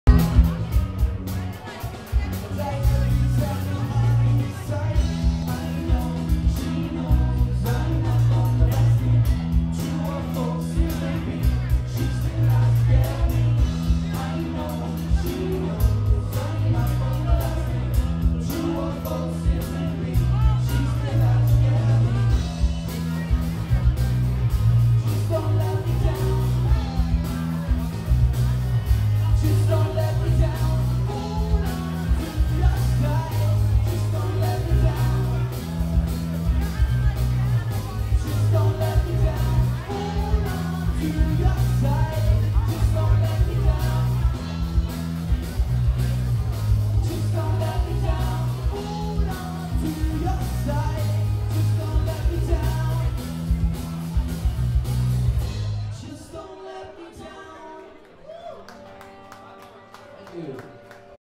band at PJ OReillys with panoramic.MOV
A band plays more at PJ O'Reilly's pub in Abu Dhabi.